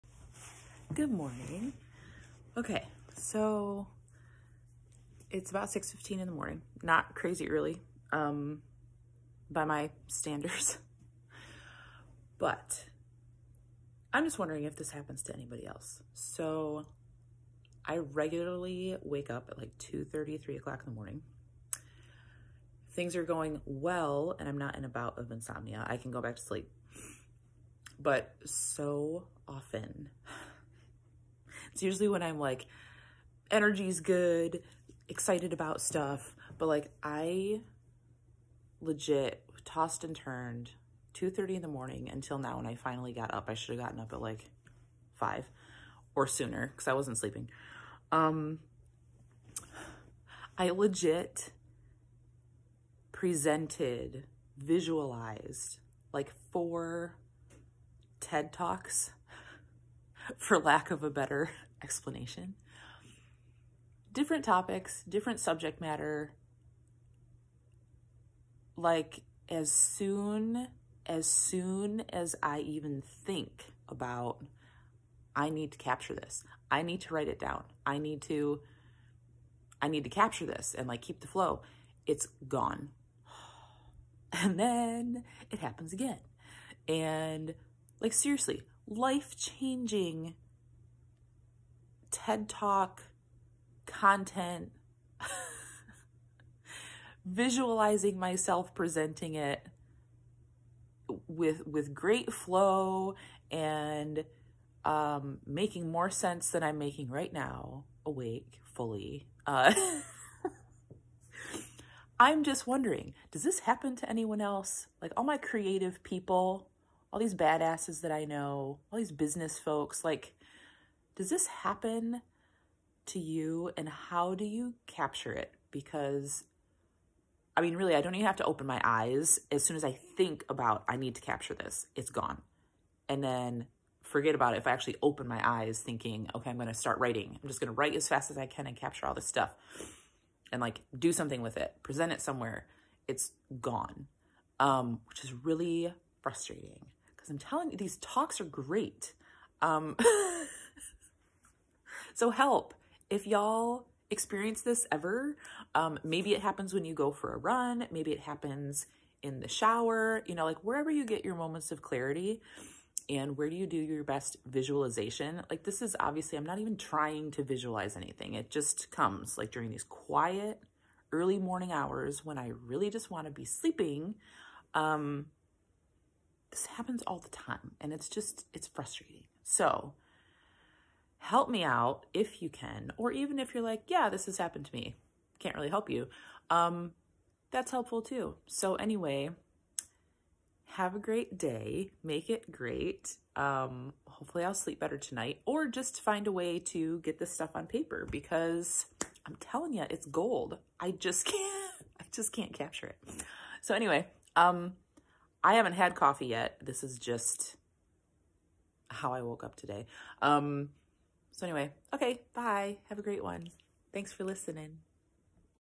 Do you have your best moments of clarity around 3am?
I give the BEST TedTalks around 3am...
In my head...
Un-intentionally Visualizing them as if they’re real.

As soon as I try to capture these ‘presentations’ through writing or recording, the flow is gone.

Can you relate to this??

How have you captured your best visualizations, ideas, thoughts?!

I need to get this stuff out of my head and actualized, but as soon as I interrupt the visualization the entire flow and content just disappear / are out of my grasp. ?! What IS that?!